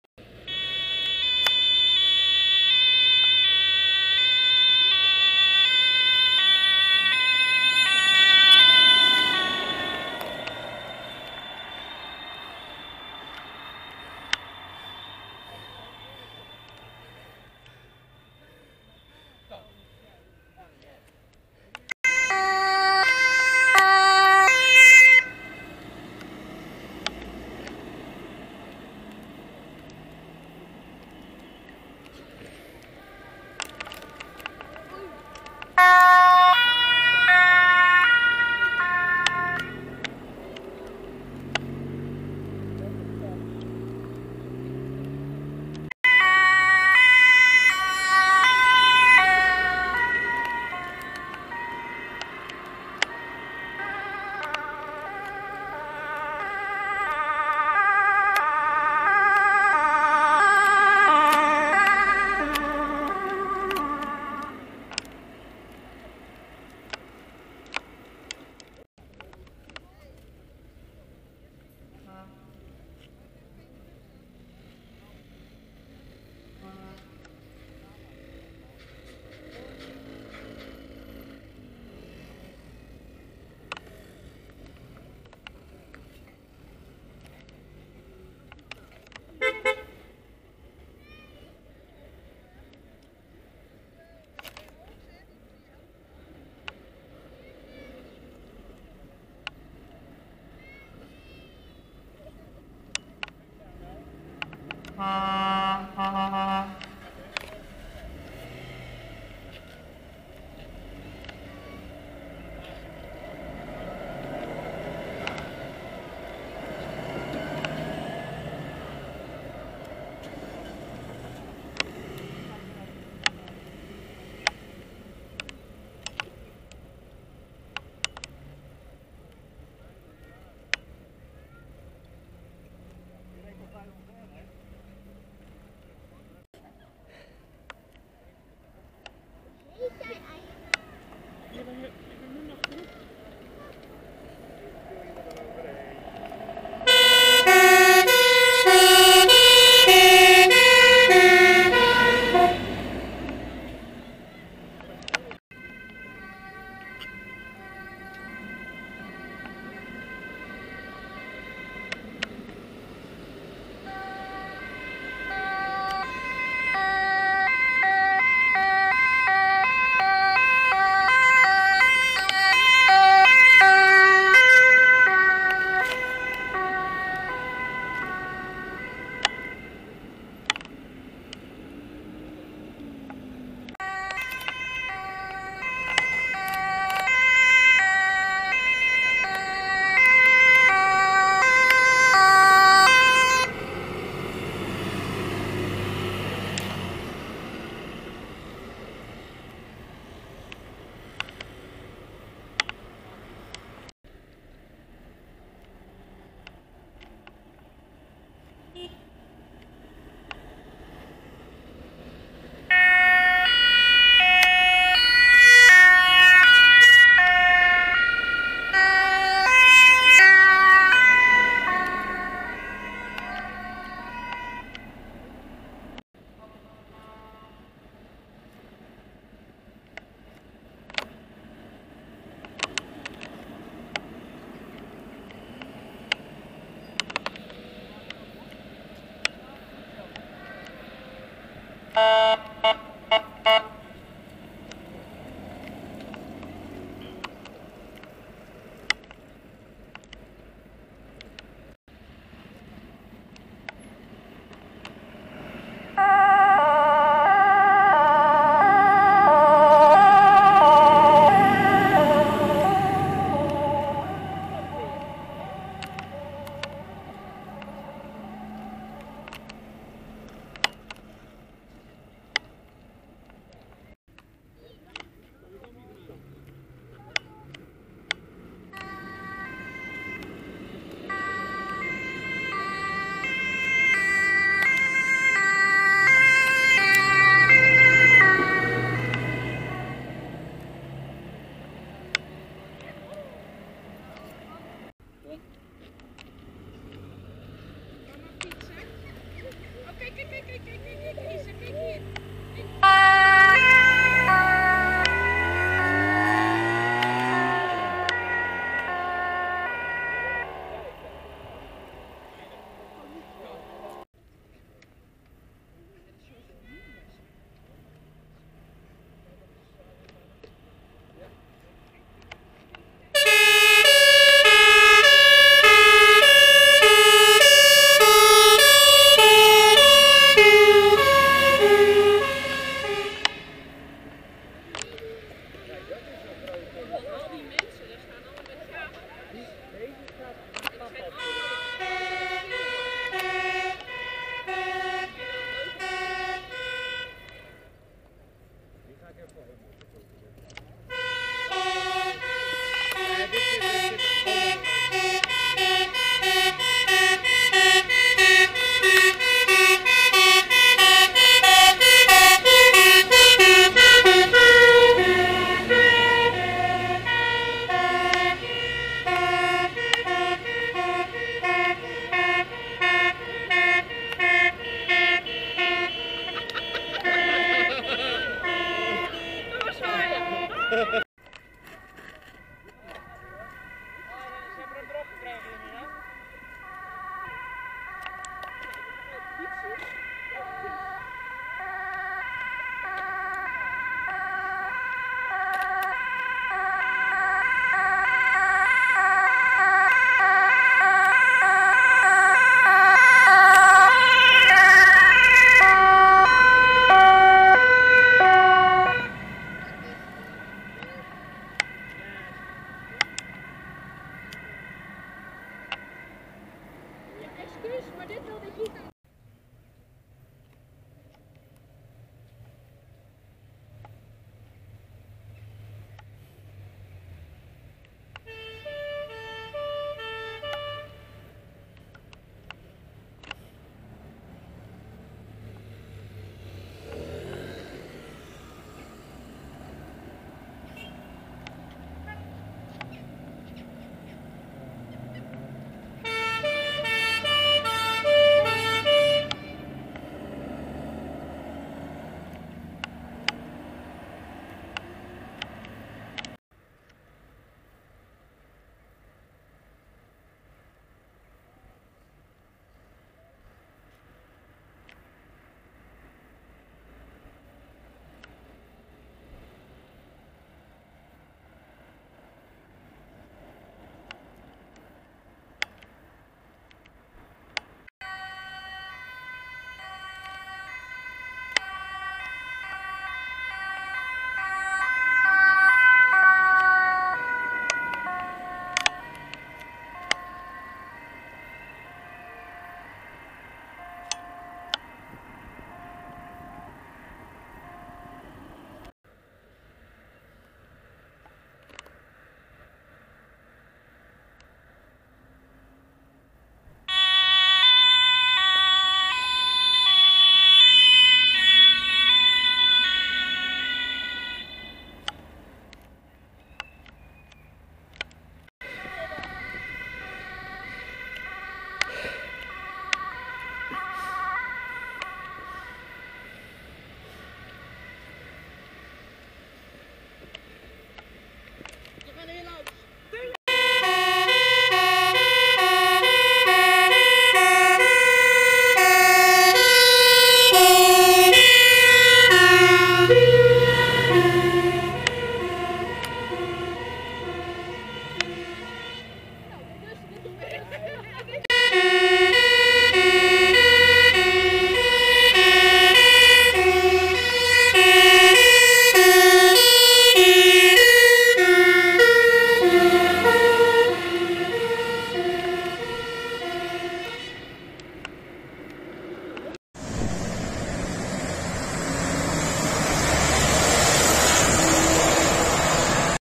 (#1607) Diverse hulpdiensten voertuigen vertrekken vanaf de hulpverlersdag in Hoorn.
te zien zijn:

0:00:00 SIV Politie Amsterdam 8230 (XG-520-X)
0:00:26 WO 10-6513 post hoorn.
0:00:46 OVD 10-6594 cluster oost Noord Holland Noord
0:00:56 WO 10-6514 Regio reserve
0:01:28 SB 10-6768 post Opmeer
0:02:16 MCU post Obdam
0:02:35 De Jonge demo ambulance
0:03:03 HV 10-6573 post Wognum
0:03:23 GGB 10-593 en ambulance 10-107 ambulance noord holland noord
0:03:46 Ambulance 10-105 en RB BGN-110
0:04:11 TS 10-5132 post hoorn centrum
0:04:33 TS 10-6835 regio reserve district west friesland
0:04:54 politie motor.
0:05:14 TS 10-6832 team west friesland regionale reserve
0:05:35 HW 28-6451 Marine Brandweer Den Helder
0:06:21 HA en materiaal bus politie amsterdam.
0:06:58 TS 744 jeugdbrandweer hoorn.
0:07:36 VW T6.1 bike bus en B-klasse politie
0:07:58 B-klasse politie
0:08:18 VW T6.1 bike bus politie
0:08:38 Mobiel Media Lab Politie DAF (92-BNR-9)
0:08:53 TS 10-5131 Post Hoorn-blokker
0:09:11 HA 10-6685 + WTS 10-6565 post zwaag.

in this video you will see:

Code 3 multiple emergency services leaving the safetyday in hoorn